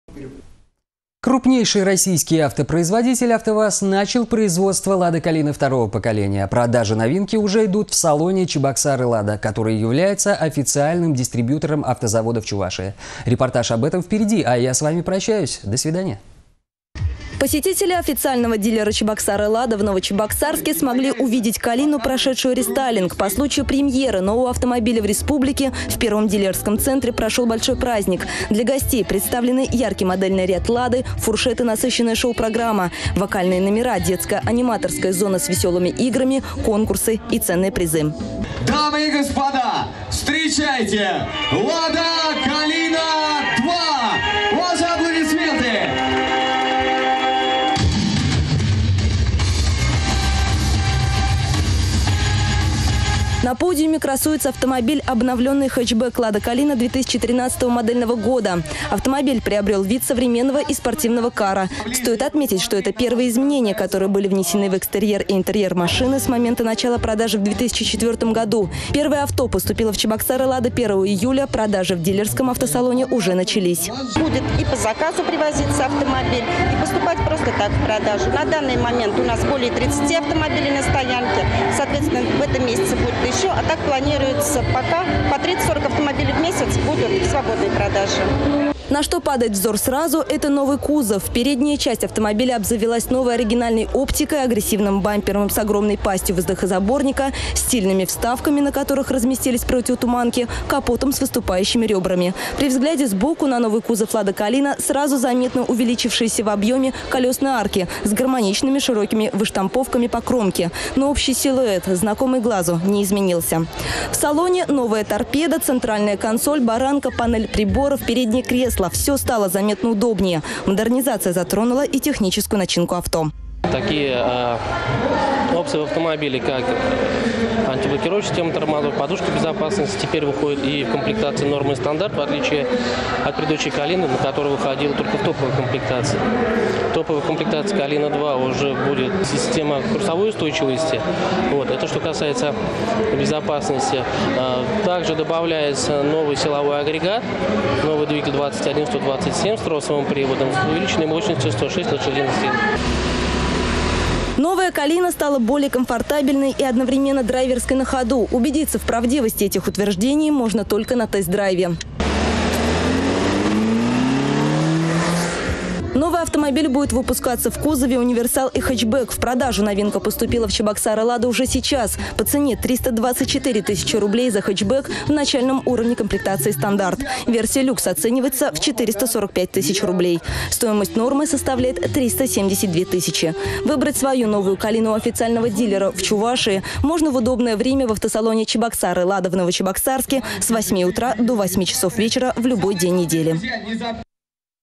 Лада «Калина» второго поколения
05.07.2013
Крупнейший российский автопроизводитель АвтоВАЗ начал производство Лады Калины второго поколения. Продажи новинки уже идут в салоне «Чебоксары-Лада», который является официальным дистрибьютором автозавода в Чувашии.